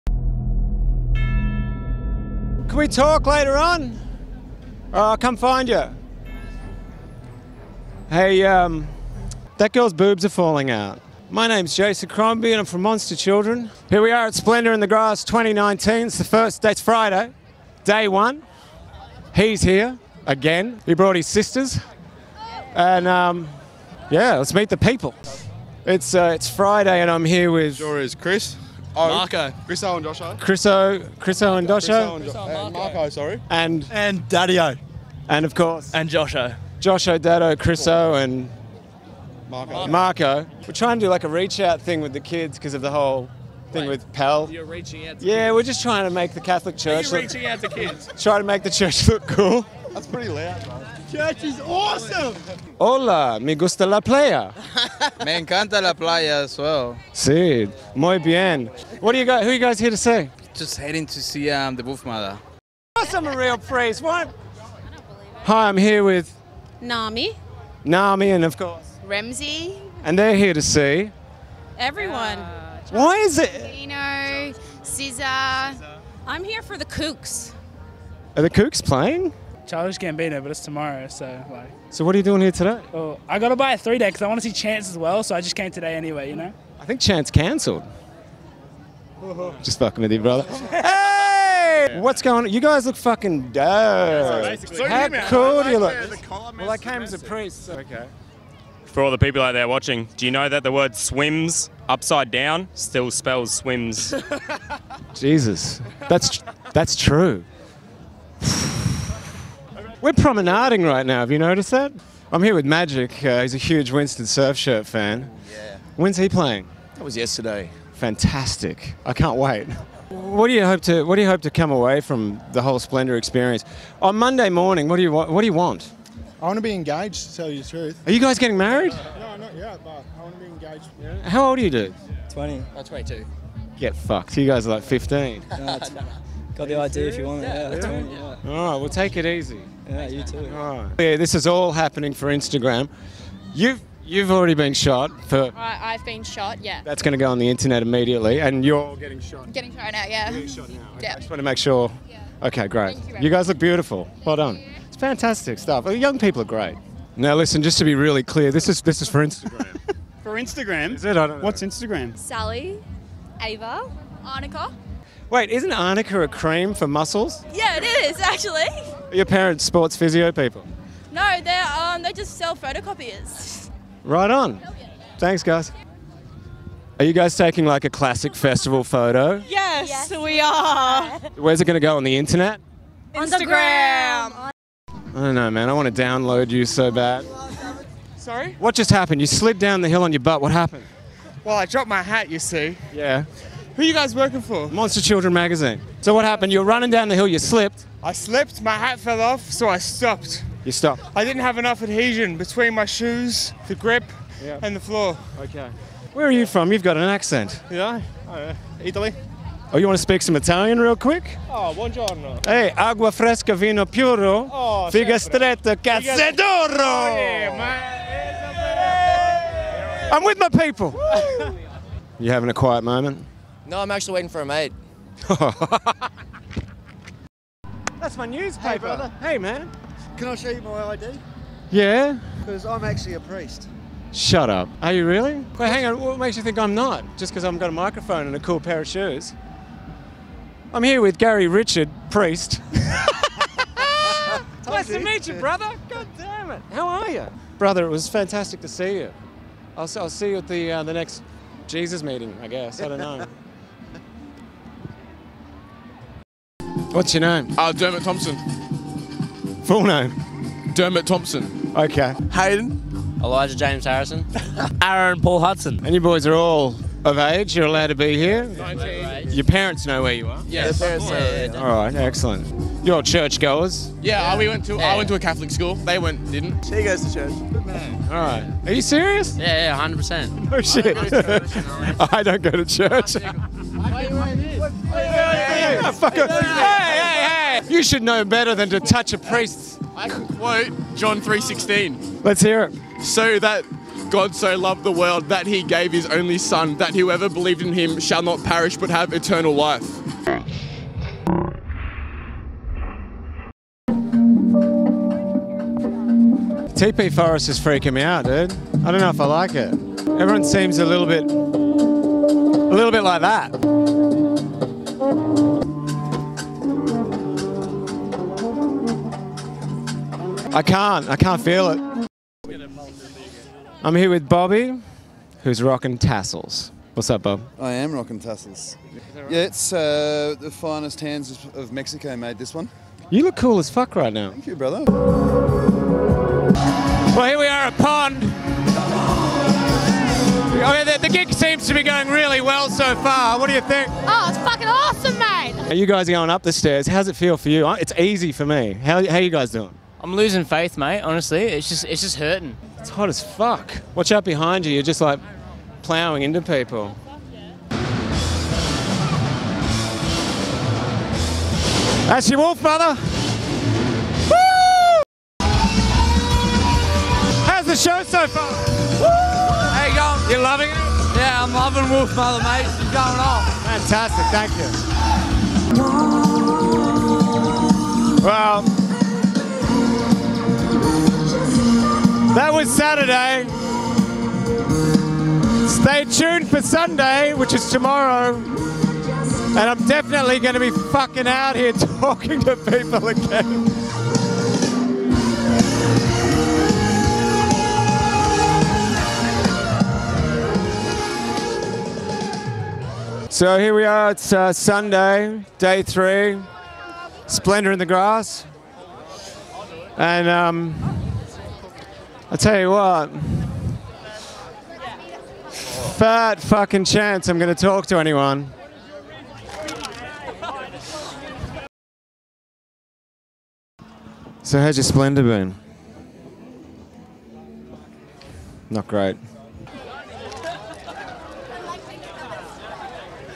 Priest 2 Meet U, Splendour!
At this year’s Splendour in the Grass, I put on a priest outfit (no reason, really) and interviewed some of my fellow festival-goers with filmmaker Tom Cole. It was a lot of fun, and I met some cool people, including an actual priest and four genuine churchgoers. What are the odds?

Filmed, edited and directed- Tom Cole & Jason Crombie